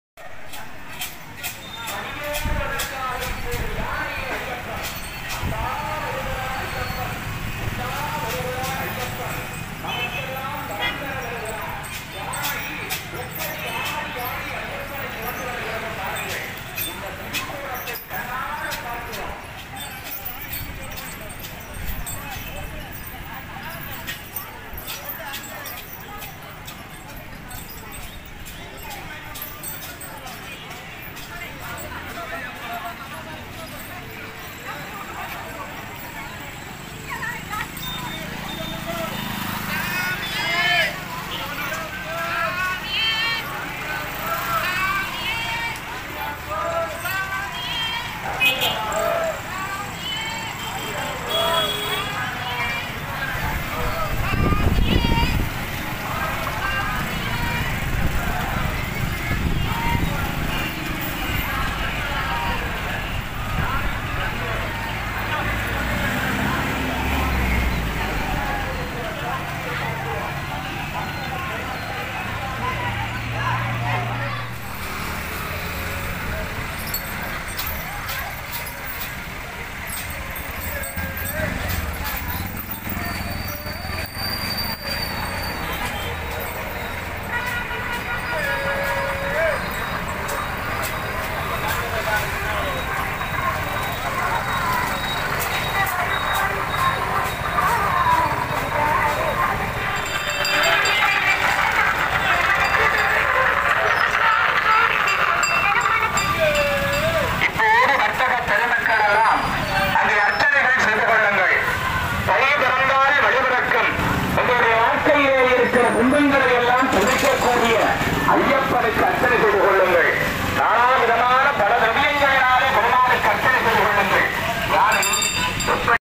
யாழ்ப்பாணத்தில் களை கட்டிய ஐயப்பன் ஊர்வலம். Jaffna Iyappan Fasting.
யாழ்ப்பாணத்தில் களை கட்டிய ஐயப்பன் ஊர்வலம். Jaffna Iyappan Fasting. Pandatharippu, panippulam Iyappan temple